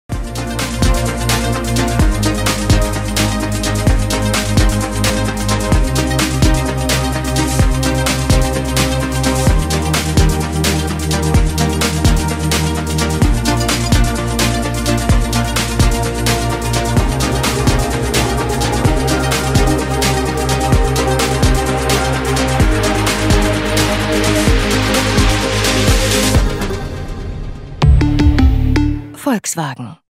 Nuevo Teramont: Elegancia, tecnología y confort.
Nuevo Teramont: Elegancia, tecnología y confort. Equipado con Side Assist, IQ Light, Park Assist, Cargador Inalámbrico y mucho más.